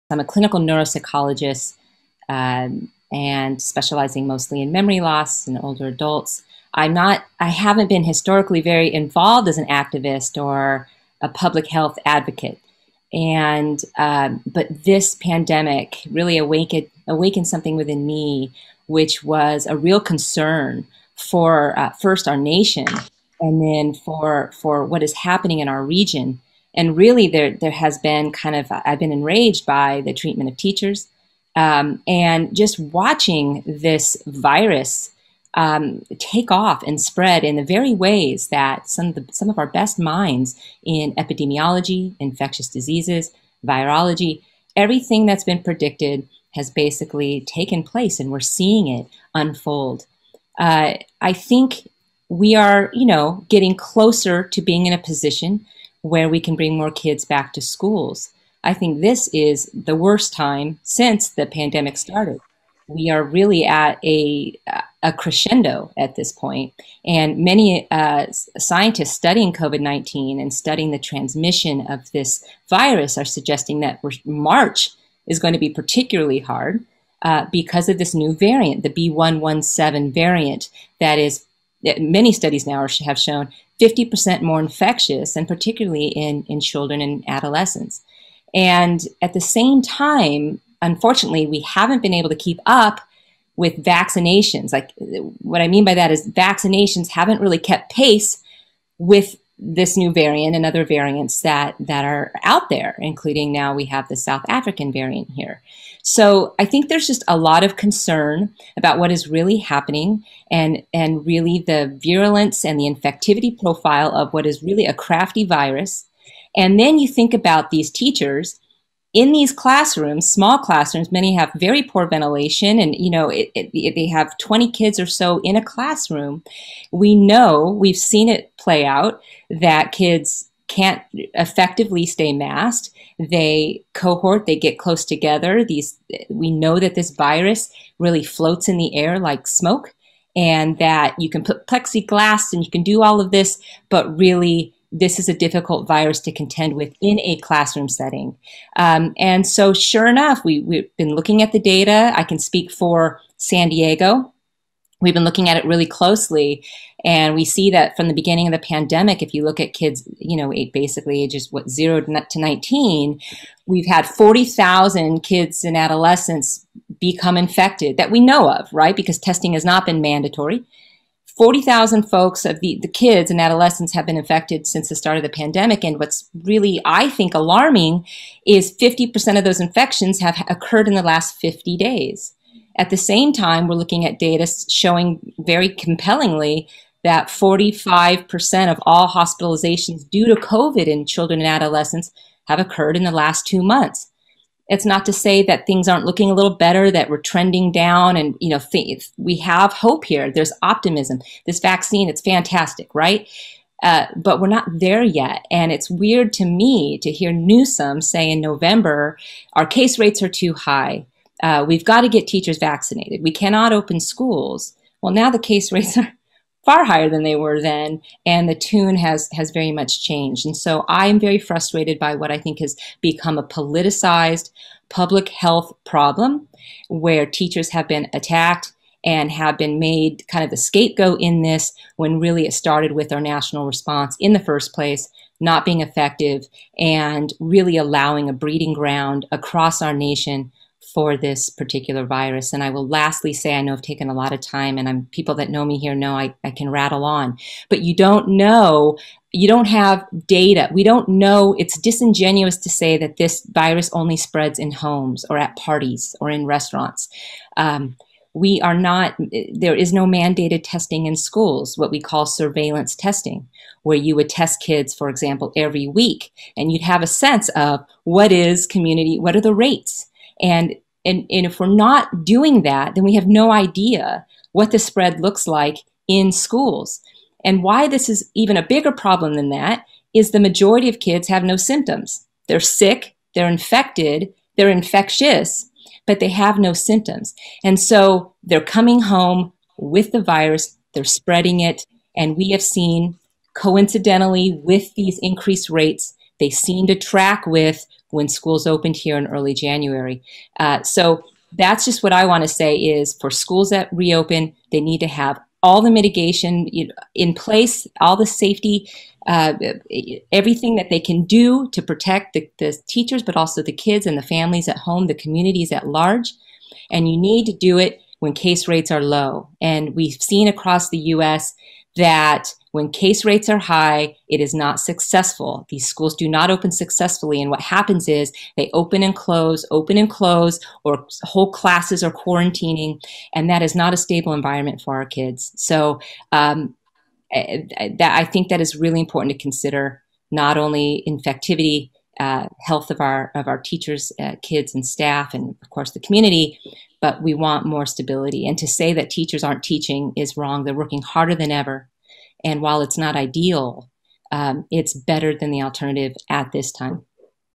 Press Conference | Sharing Reopening Concerns to Lawmakers – Clinical Nurse/Psychologist
San Diego parent & clinical nurse/psychologist Lisa Delano-Wood penned an op-ed about her concerns regarding reopening schools when it is not safe to do so.